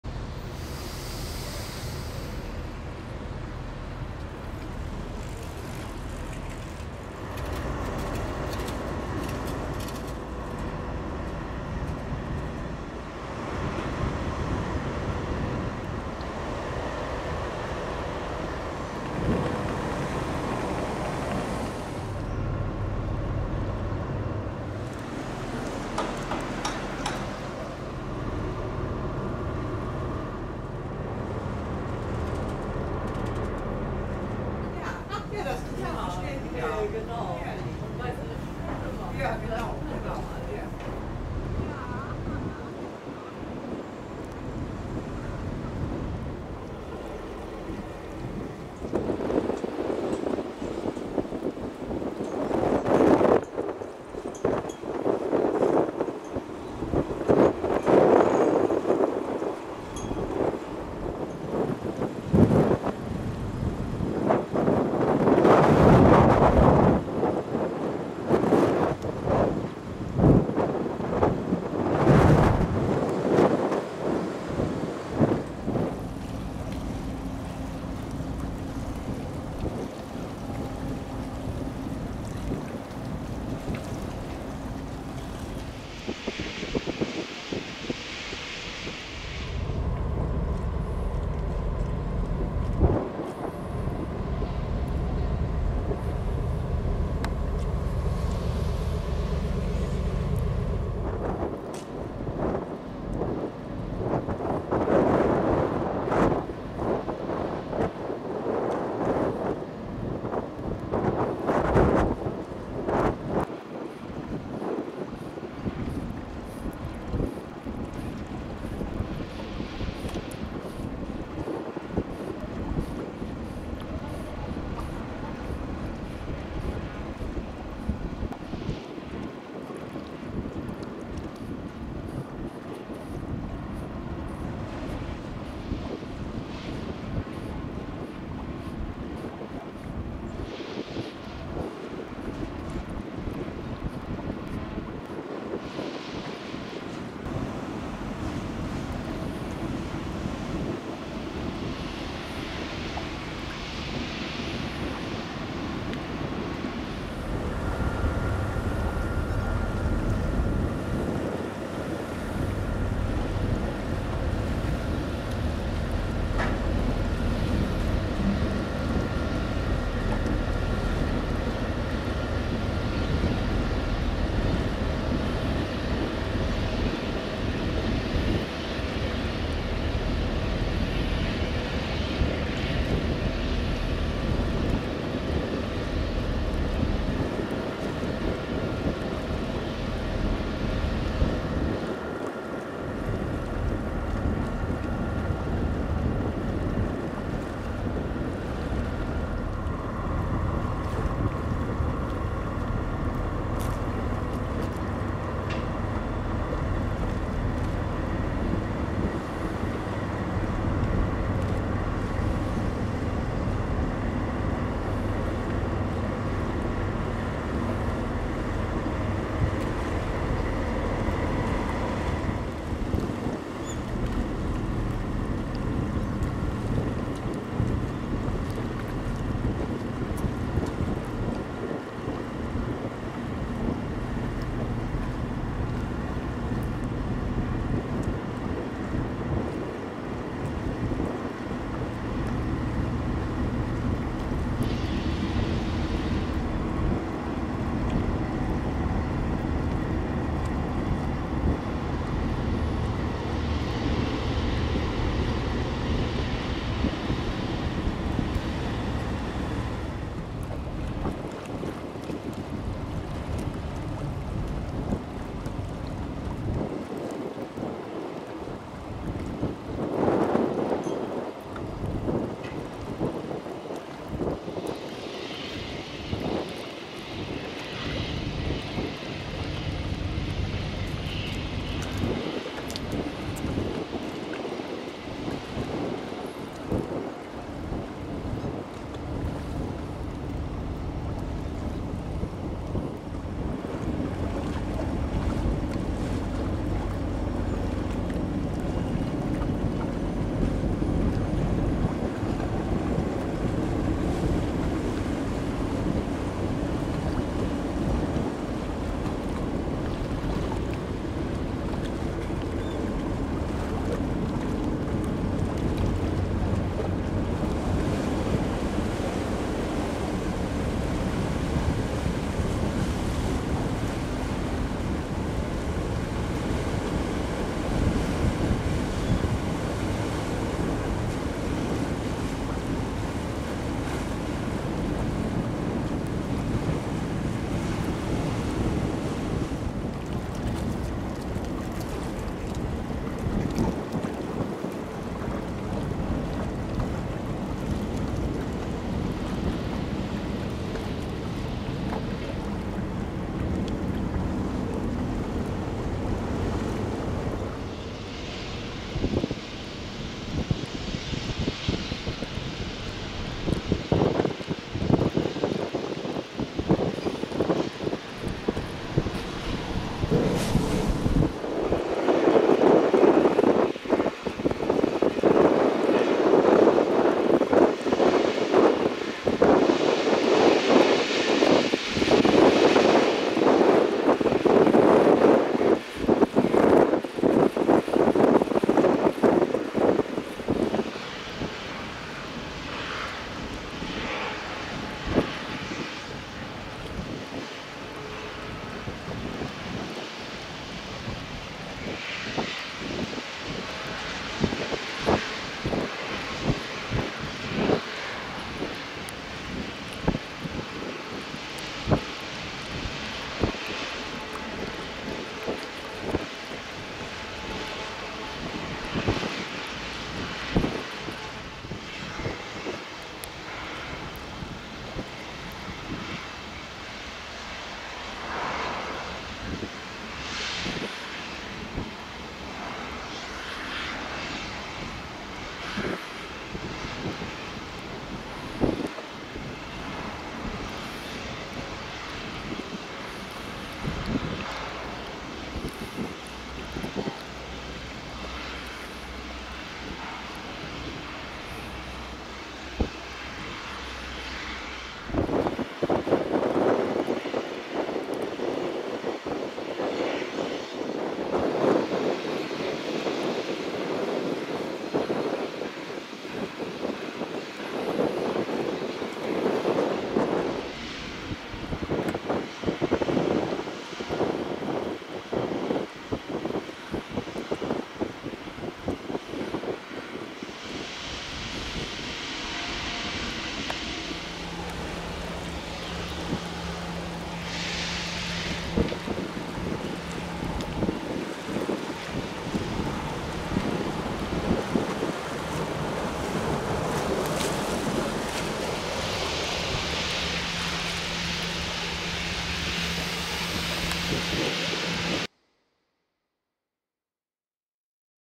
"GERA"
Am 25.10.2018 verholte die "GERA" mit 2 Schleppern zur "BREDO WERFT" im Bremerhavener Fischereihafen.